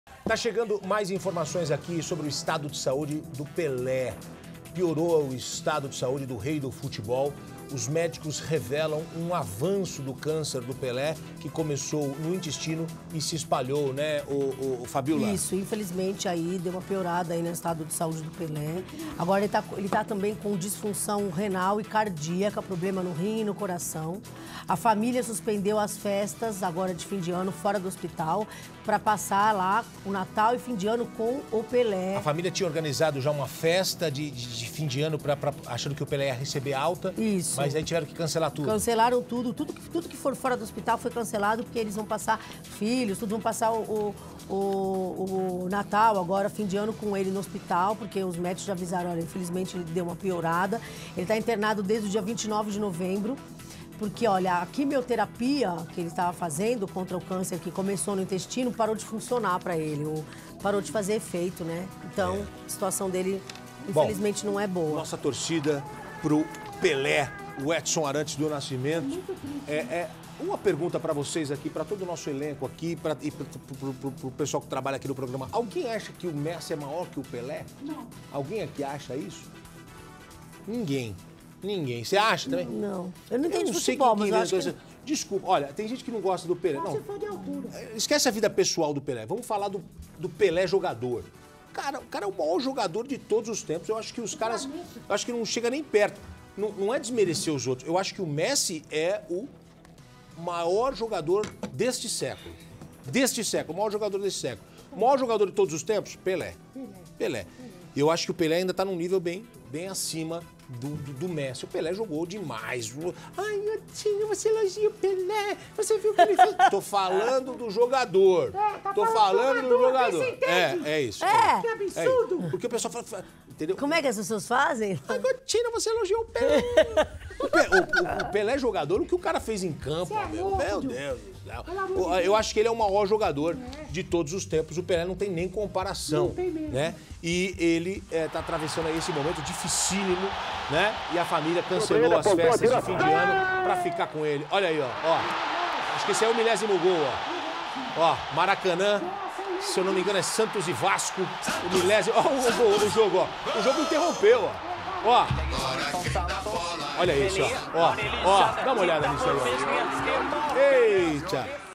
Família de Pelé cancela festas de fim de ano para ficar no hospital
A família de Pelé suspendeu as festas fora do hospital para passar o Natal e o Ano Novo com o ex-jogador. Os médicos revelaram um avanço do câncer de Pelé, que começou no intestino e se espalhou. Ele está com disfunções renal e cardíaca.